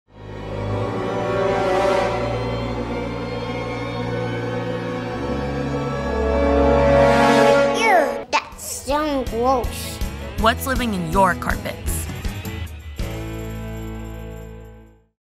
That's So Gross Organic Rot
Did you know… carpet absorbs organic matter that sinks and rots.

Did you know that your carpet acts like a secret ecosystem? Not only does your carpet absorb dirt and dust, but it also becomes a cozy home for organic matter that sinks and rots. Just like a mini compost pile, your carpet becomes a natural filter, trapping everything from dead skin cells to pet dander. Over time, this organic matter can build up and become a breeding ground for bacteria and allergens. That's why regular carpet cleaning is essential!